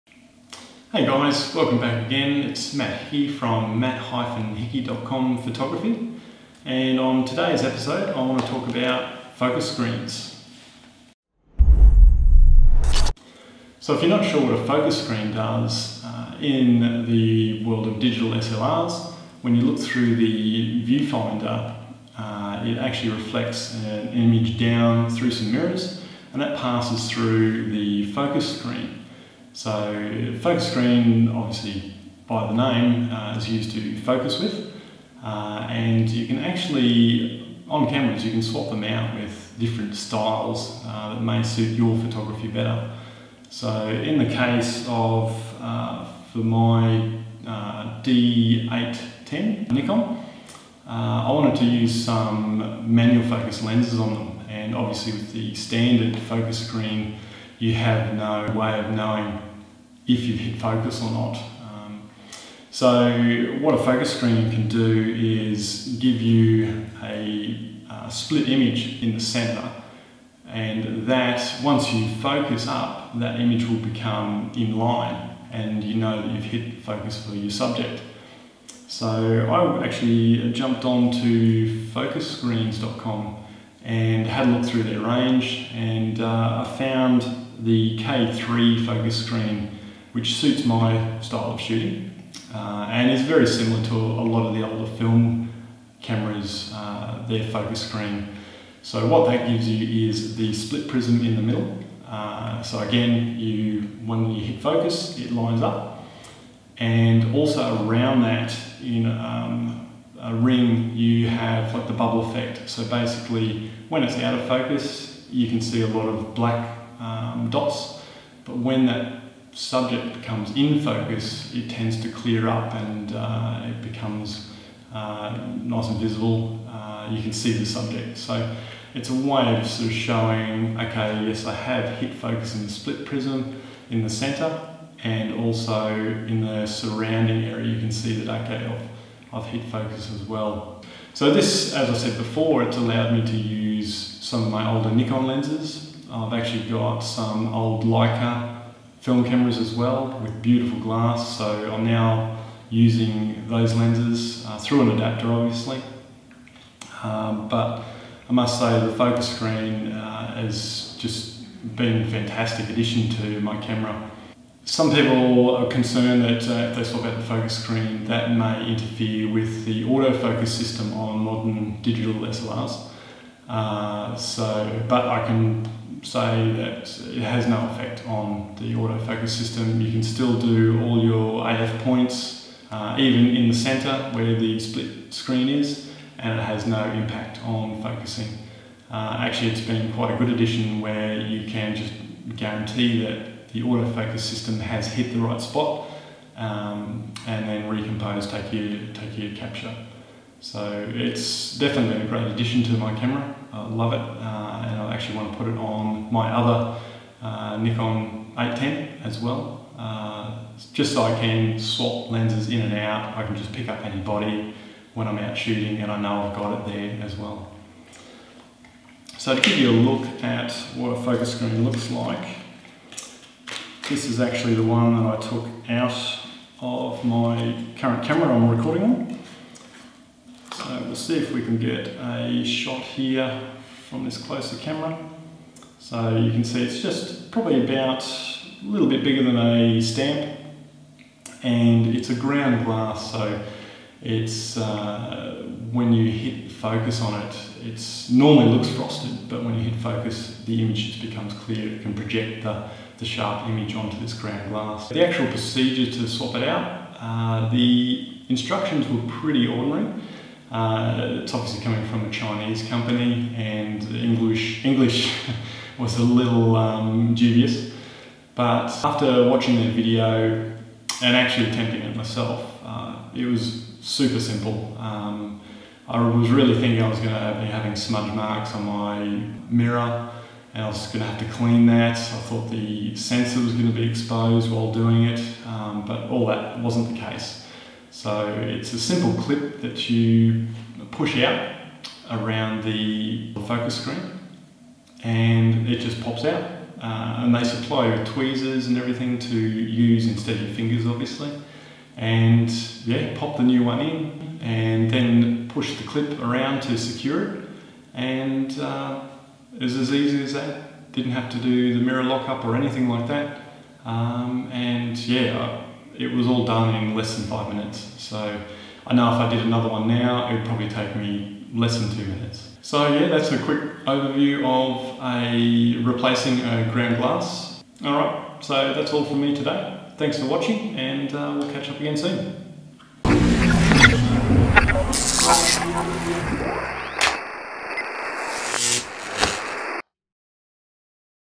Why use a Focus Screen on a DSLR?
In this episode, we explore why you would consider replacing your standard focus screen in a DSLR for a custom focus screen and any potential issues.

Timeline:
0:00 - Introduction
0:15 - What is a Focus Screen?
0:45 - Types of Focus Screens
1:08 - How a Focus Screen works
1:30 - The K3 Focus Screen
2:20 - What I can now do with a Focus Screen
2:45 - Auto-Focus system with a custom Focus Screen
3:40 - What a Focus Screen looks like
4:15 - Procedure to swap out a focus screen